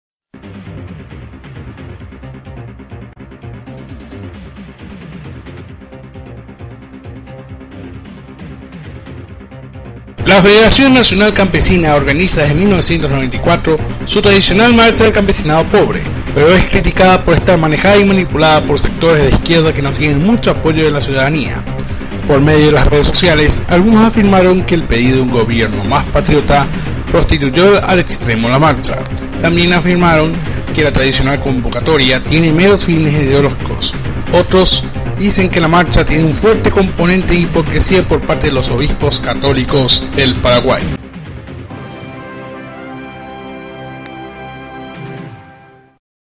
SemanaVD1: MARCHA DEL CAMPESINADO POBRE, CRITICADA EN REDES SOCIALES
La Federación Nacional Campesina, organiza desde 1994, su tradicional marcha del campesinado pobre. Pero es criticada por estar manejada y manipulada por sectores de izquierda que no tienen mucho apoyo de la ciudadanía. También afirmaron que la tradicional convocatoria, tiene meros fines ideológicos. Otros dicen que la marcha tiene un fuerte componente de hipocresía, por parte de los obispos católicos del Paraguay.